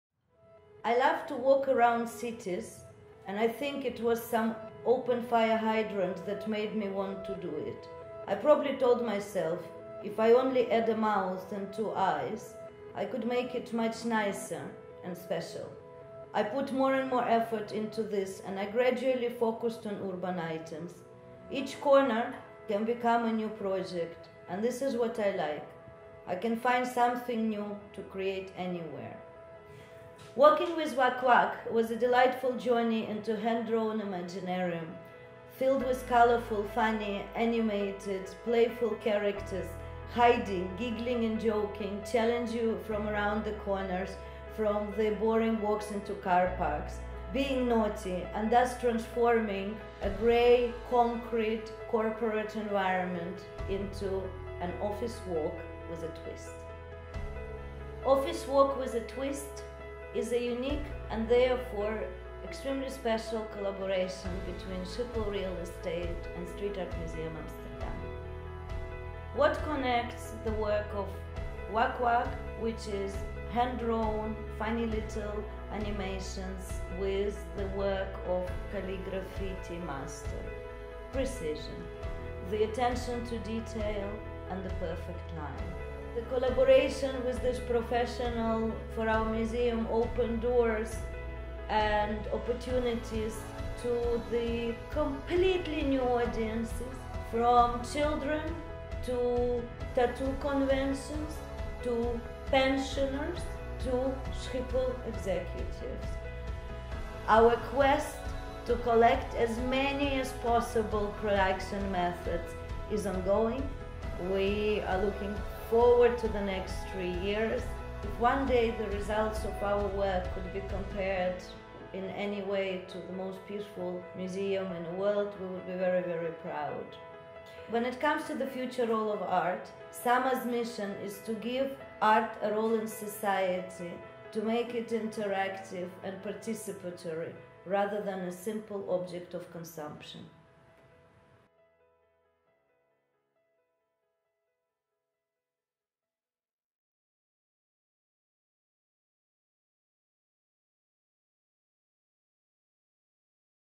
Street Art Museum Amsterdam - Office Walk with a Twist (Schiphol/OakOak)
At Street Art Museum Amsterdam we are marvelling at this amazing opportunity to use New Street Art form as a catalyst to start a dialogue between people and entities that otherwise would never talk.

The art of OakOak is first of all intelligently funny. It takes you by surprise and ignites the imagination with its playful, colourful, graceful and extremely sharp simplicity. Secondly, the artist is an expert at carefully measuring locations turning each little masterpiece into a slow cooking alchemistry of engagement. OakOak is a collector’s dream for he draws all of the artworks by hand without any use of technology, thus creating an ultimate original matter without the possibility of duplication.

“Hard werken kun je ook in een grappige omgeving!” Hank, Schiphol Employee

Campaign stops on 1 June 2016 and all the artworks will be removed by 10 June 2016. Catch it whilst you can!